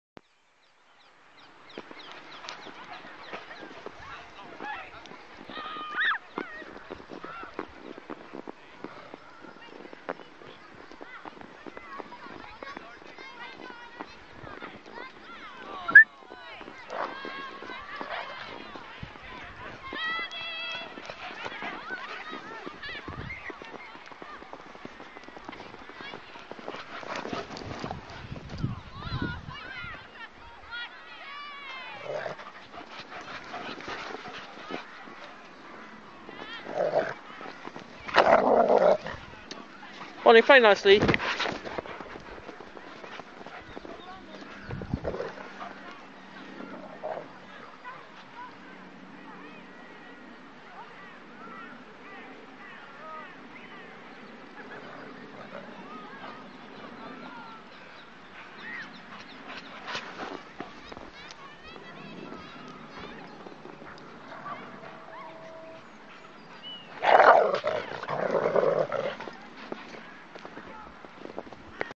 2 dogs playing in the park in the snow in London !
Play fighting in the snow.   -

Thankyou. ***

If you like this video or any on my channel please consider making a Bitcoin donation, they are gratefully accepted to: 19hyum5jc4QpX9zPaYELtEys4umaL4aKhF